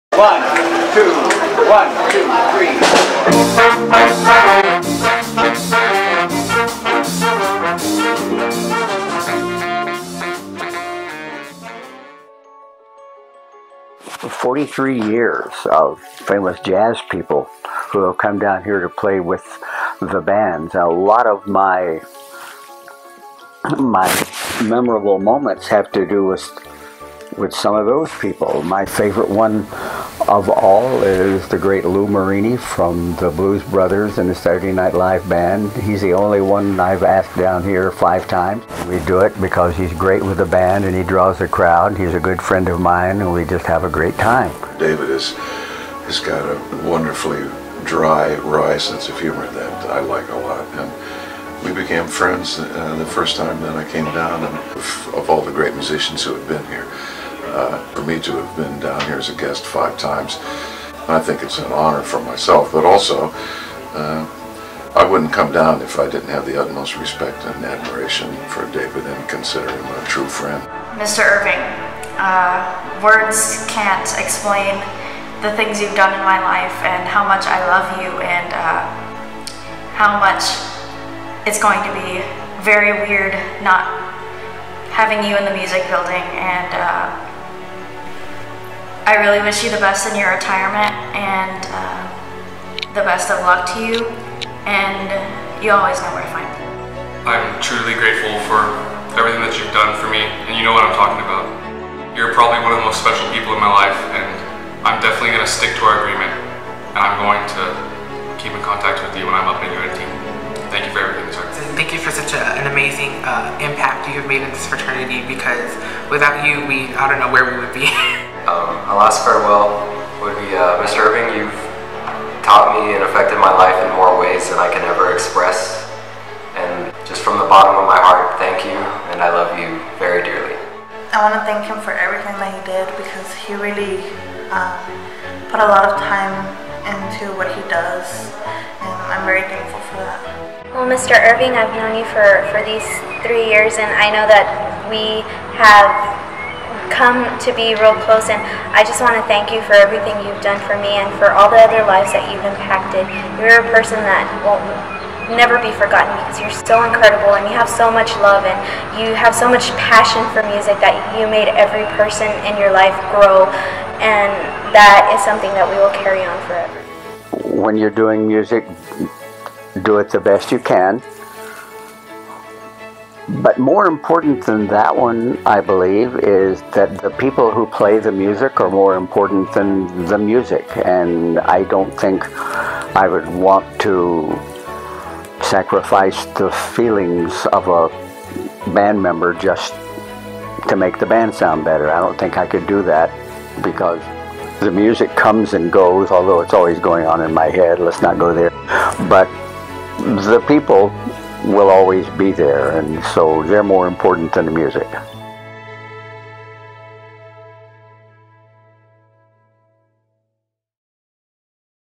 Farewell David Irving
After 45 years teaching in the Music Department at Del Mar College, David Irving has made a lasting impression on the Del Mar community, impacting students by showing them how to follow their dreams and inspiring staff and colleagues. Del Mar College wishes professor David Irving a happy retirement and bon voyage!